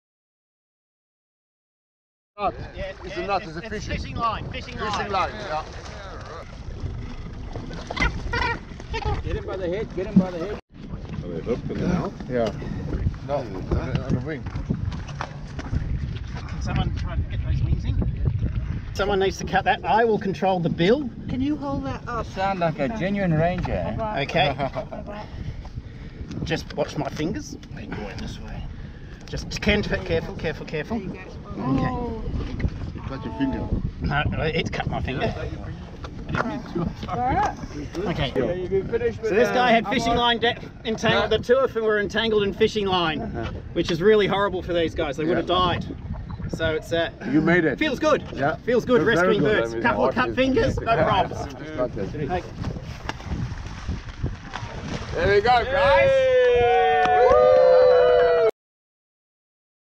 Ocean Sea Bird Rescue — South African Sardine Run
While out on the sardine run off the coast of South Africe, we were alerted to two particular birds by their voices—very different from the other birds above us who were diving and searching for sardines. Luckily one of this years’ team members, Jono Stevo, was a ranger from Australia. His knowledge was crucial at this moment because he realized the sound we were hearing was not one of happiness, but one of distress.

I asked Walter our skipper to alter the course of the boat to go in the direction of the birds’ sound. As we arrived all of us immediately diagnosed the problem—the two gannets’ beaks were tangled up in a man-made fishing line; they were stuck together—unable to fly, unable to function.

Normally, we will not always intervene in the wildlife process and survival; however, this was a human-made problem that needed a human-made solution; we were happy to assist.

Walter and Jono immediately took control of the situation, easily getting towels over the birds, and effortlessly pulling them from the water onto our boat. Jono, with his deep experience rescuing other wildlife, was quickly able to calm down the birds and remove the fishing line that was threatening them. The only minor injury to anyone was a small cut on his finger, and in very short order, we were able to set the birds free.

As we watched the gannets fly away, everyone onboard felt relief and joy in seeing the two beautiful birds able to return to their lives — thank you Jono, Walter and everyone on board for your participation in the rescue effort.